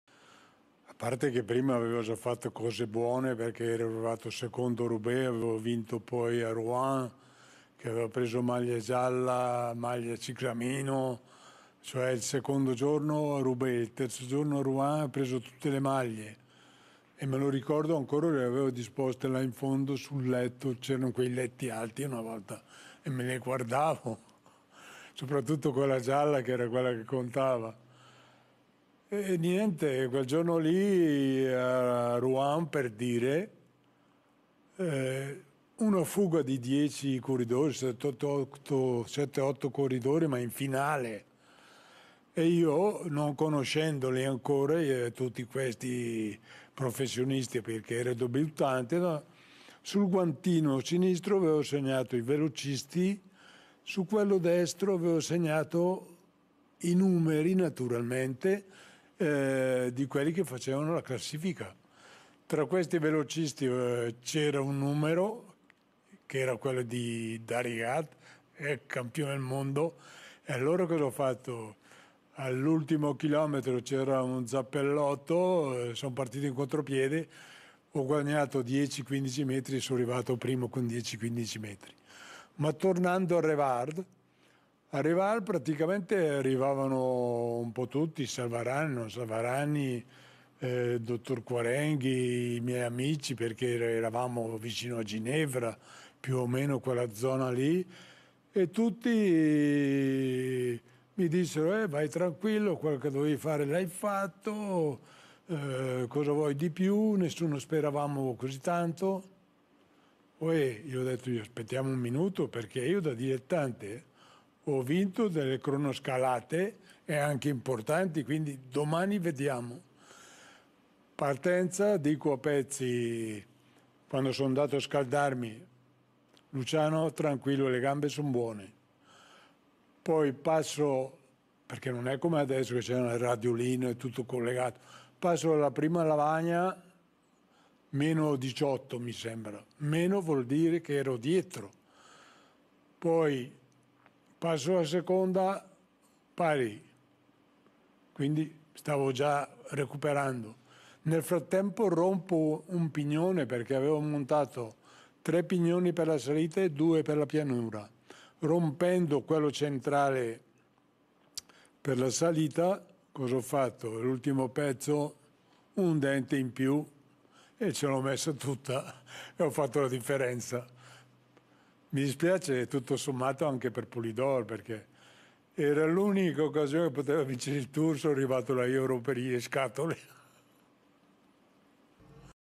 Gimondi Tour 65
Il racconto di Felice Gimondi delle tappe decisive del Tour '65